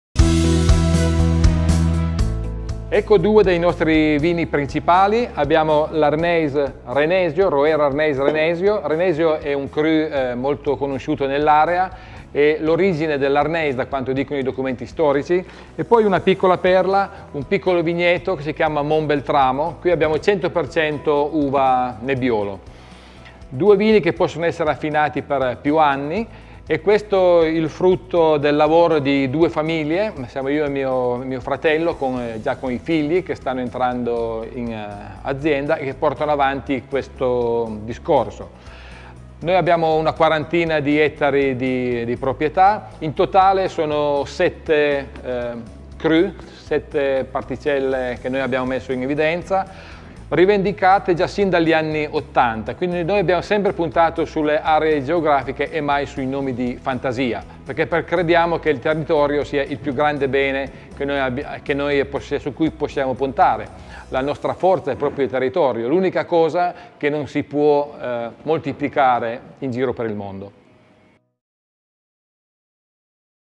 A sip of Roero DOCG: Malvirà - Italy
Love for the land and for wine are the factors that have driven three generations
in Malvirà to put the “soul and spirit of Roero” into the bottle, with the aim of
letting you discover it sip by sip.
The third generation of the Damonte family is now at the helm of the company,
to steer it towards the future, maintaining the capacity for producing wines that
reflect the land in its purest and most elegant form.

[Visual: video langhe.tv]
CAMPAIGN FINANCED ACCORDING TO EU REGULATION N.1308/2013